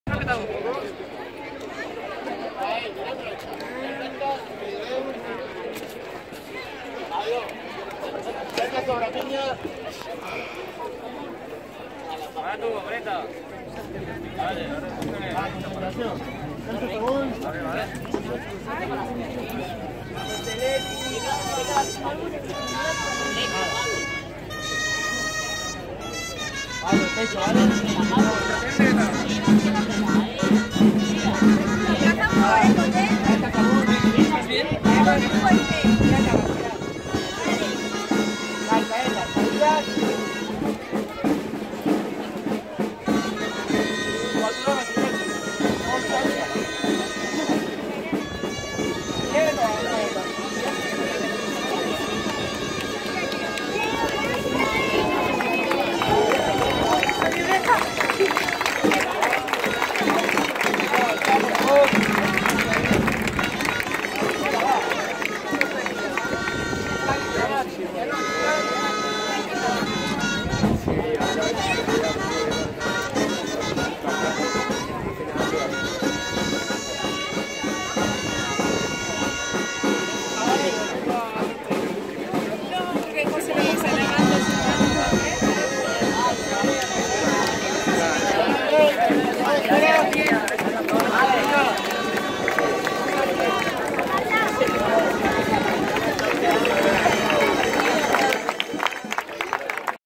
4xP4 - Diada de Tardor a Castelldefels
4xP4 dels Castellers de Castelldefels a la Diada de Tardor al Castell de Castelldefels, amb els Castellers d'Andorra i els Xiquets de Vila-Seca.